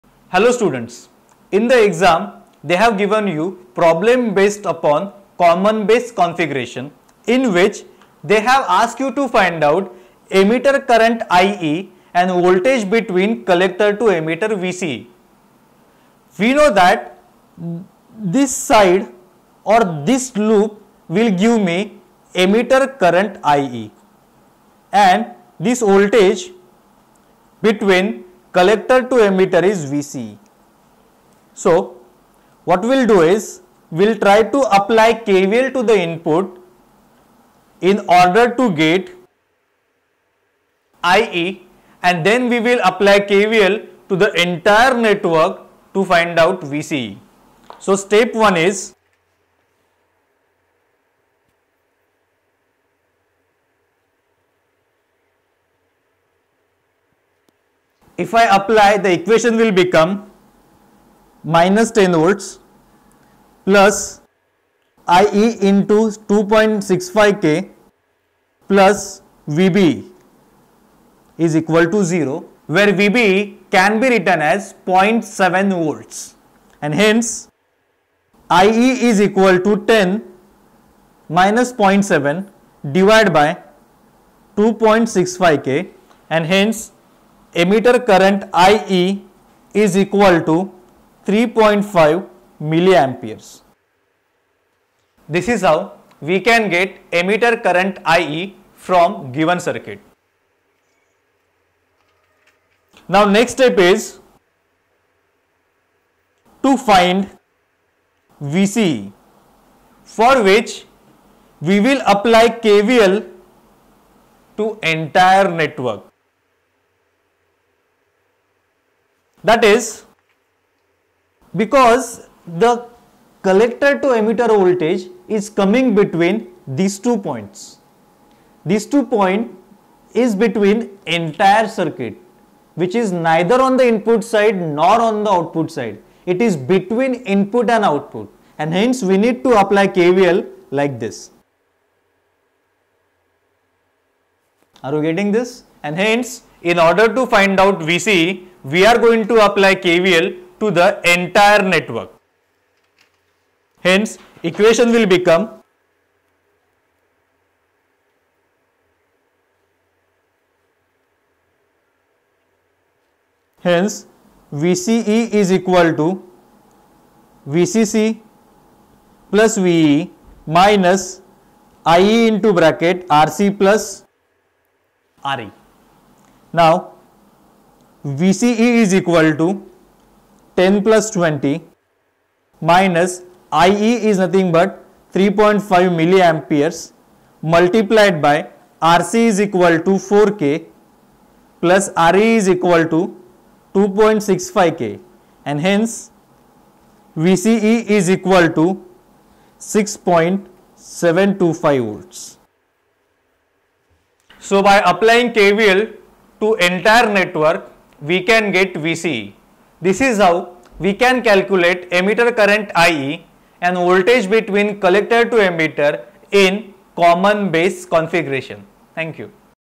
Common Collector Configuration Problem 2 in DC Analysis - Bipolar Junction Transistor
Video Name - Common Collector Configuration Problem 2 in  DC Analysis

Chapter - Bipolar Junction Transistor

Happy Learning!